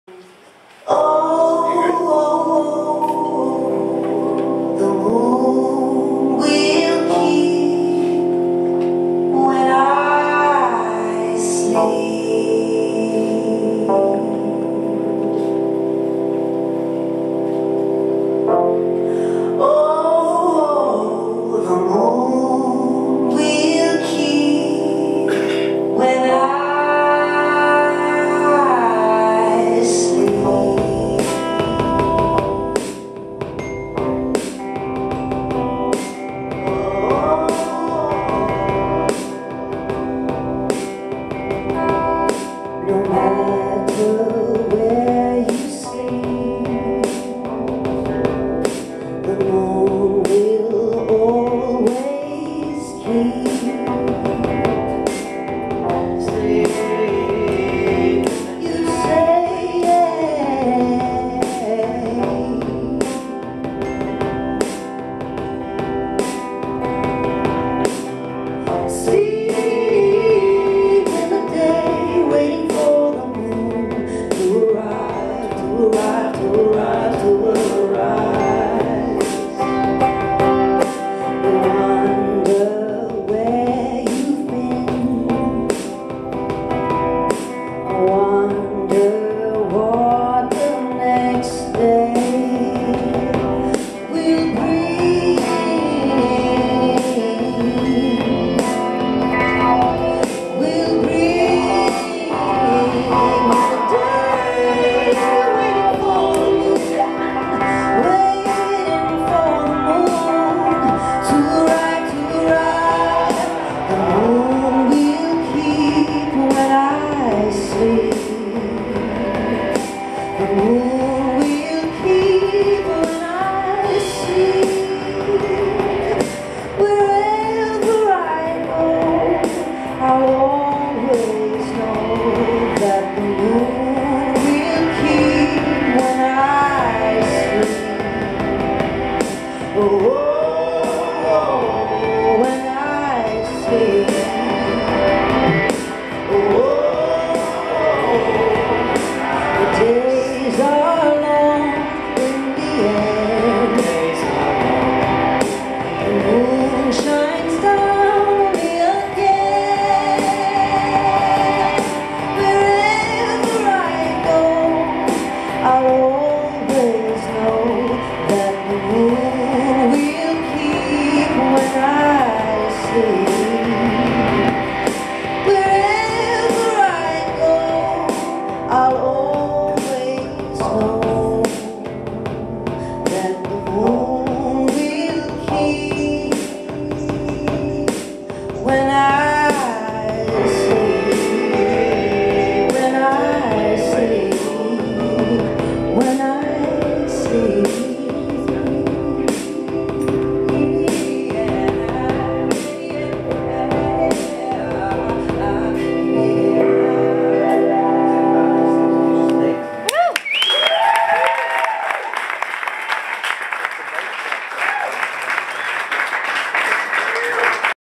Daughter of Dynamite
Performed at "Television is Dead" night, 107 Projects, 107 Redfern Street, Redfern, Sydney, Australia, Thursday 18 October 2012. Background projections by VJ Hedonics.